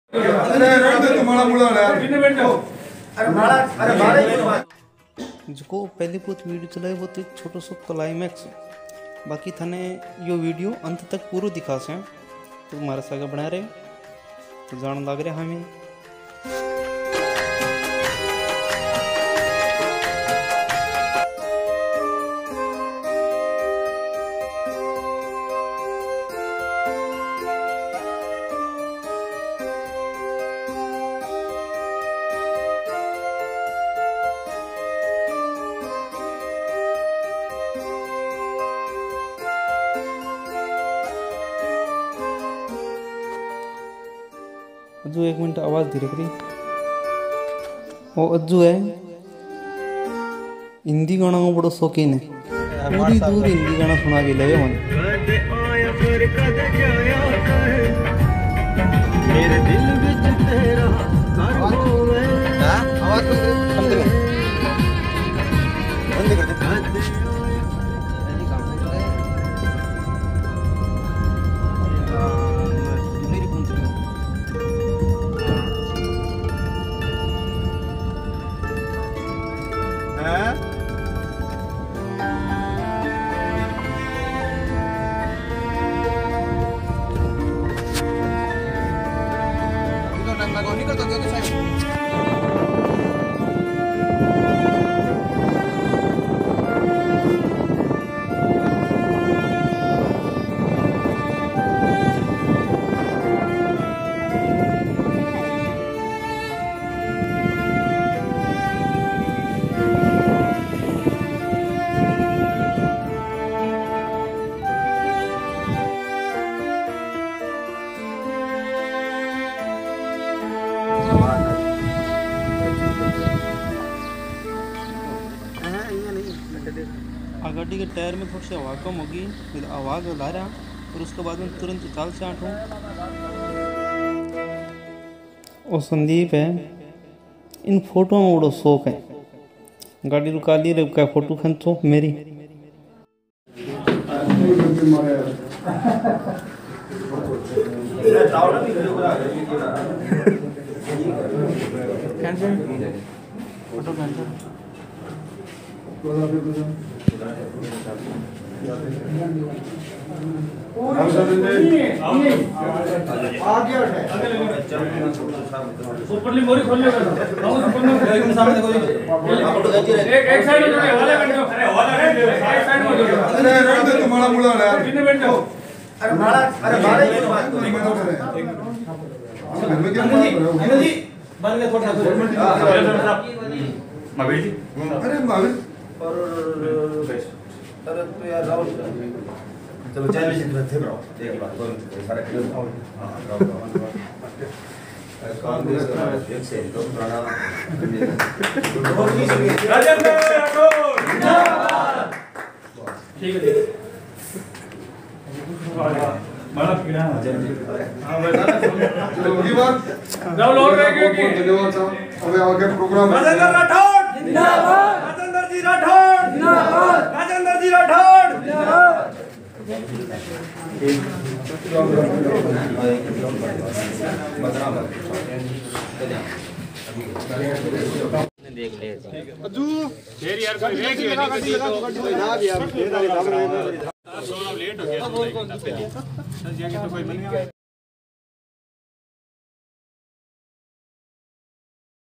Rajendra Rathore उप नेता प्रतिपक्ष (रेडी-भुरावास-धीरवास) | Taranagar || Churu
rajendra singh rathore
 rajendra rathore news

individual faces in the video
Rajendra rathore(MLA-churu}
Mahaveer bhambhu
Ajay soni
Sandeep goswami
Vinod bhambhu
Chanan godara
Harisingh goswami
Jeetmal kherwa

location
redi-bhurawas (taranagar)

record video time
13 august 2022